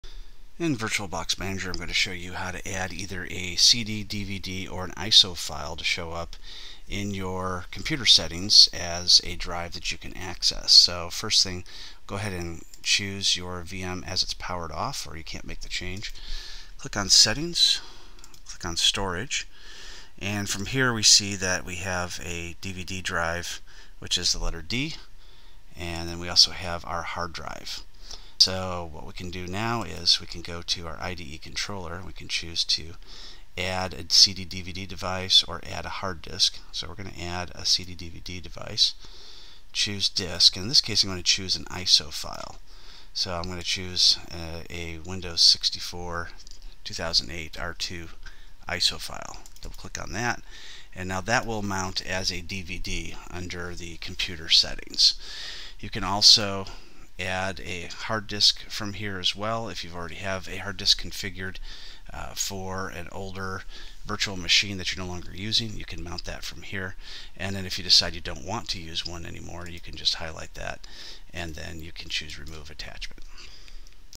How to mount an ISO file as a CD-DVD drive in a virtual machine on VirtualBox manager
Author and talk show host Robert McMillen explains how to mount an ISO file as a CD-DVD drive in a virtual machine on VirtualBox manager.